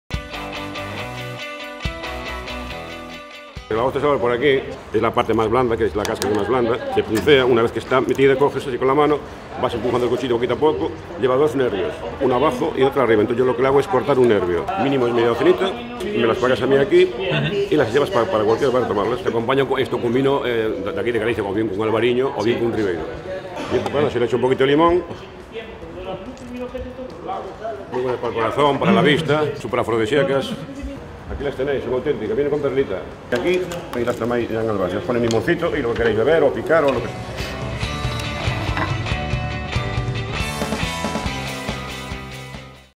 La ostra de As Rías Baixas. Hambre de Experiencias.
“A Pedra” es el nombre popular por el que se conoce la célebre Rúa Pescadería en Vigo. Allí puedes degustar las ostras más frescas de la ría a precios populares y acompañarlas de un albariño a la hora del aperitivo. Juan Manuel Maestu, ostreiro de “A Pedra” nos explica las propiedades de las ostras y nos recomienda la manera de disfrutar de su sabor como un auténtico experto.
Disfruta de la gran despensa de productos del mar y sorpréndete de la antigua y arraigada tradición marinera, marisquera y conservera de Vigo.